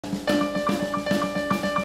ダイナミックフォト習作
カシオコンパクトデジカメ EX-Z400のダイナミックフォト機能の習作です。
動くものの画像を切り取って，別の背景画像と合成することができます。
カレンダーのサハラ砂漠写真をバックに，歯みがきロボットが動きます。